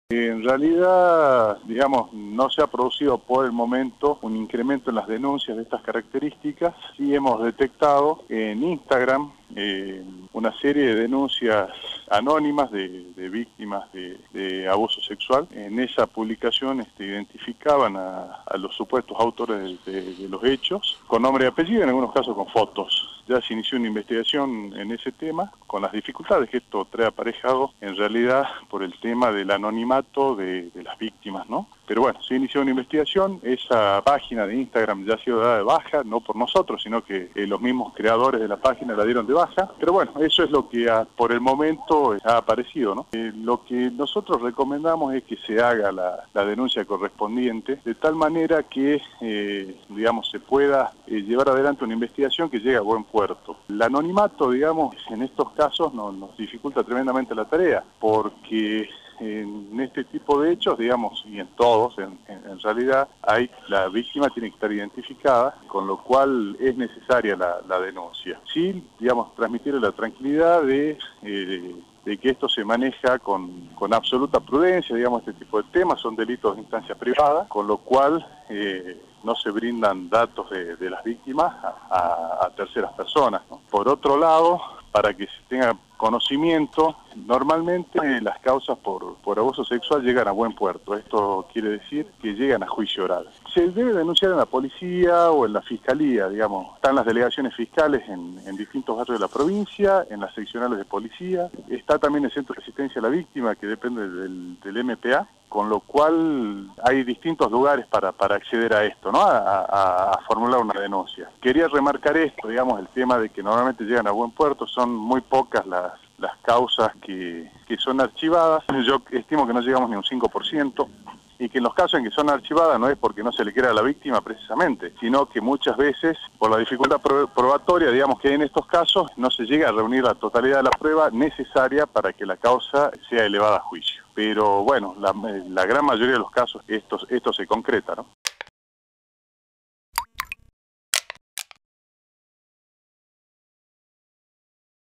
La Justicia jujeña investiga los casos de abuso denunciados en Instagram
El fiscal especializado en violencia de género explicó que la tarea es compleja por el anonimato de las víctimas.